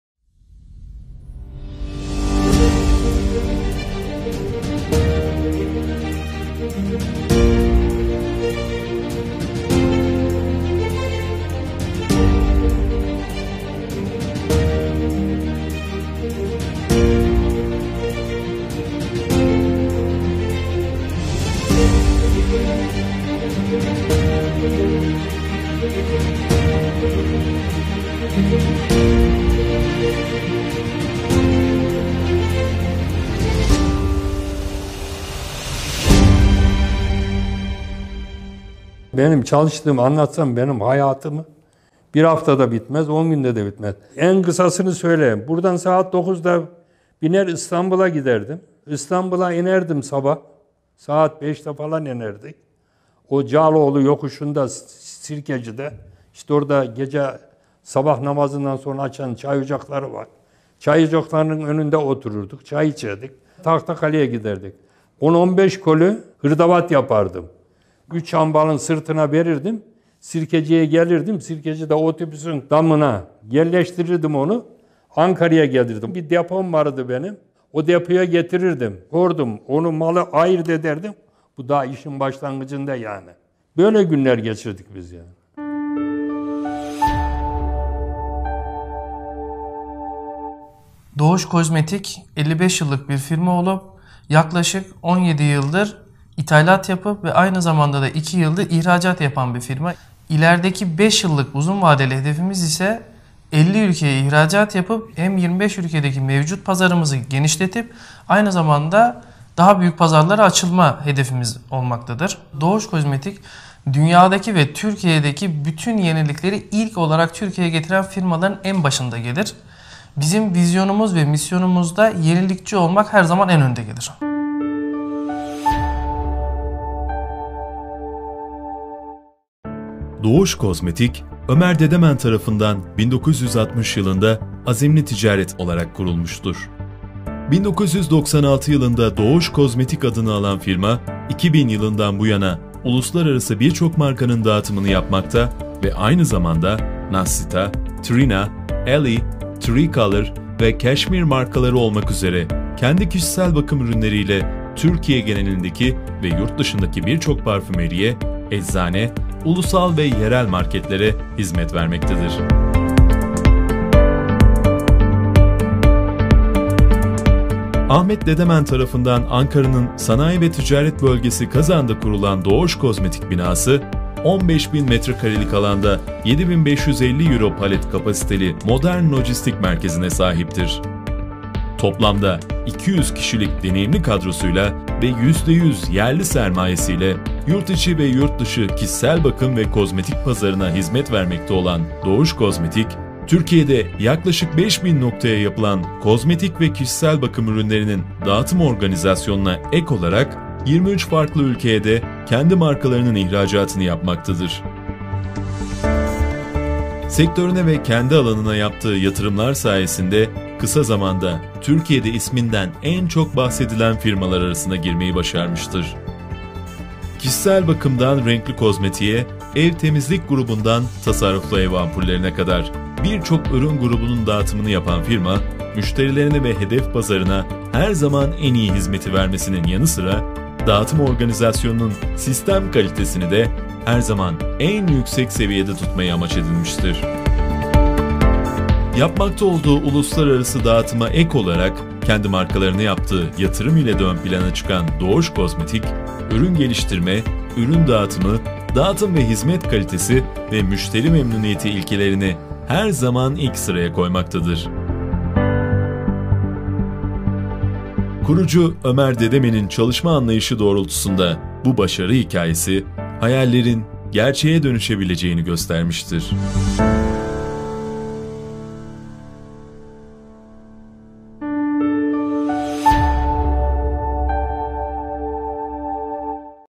Doğuş Kozmetik Inc. Corporate - Promotion Movie (English Subtitle)
Dogus Cosmetic Company gives the best service to Turkey with the 180 experienced staff in the area that have 15.000 square meter and 7550 euro pallet capacity in Ankara.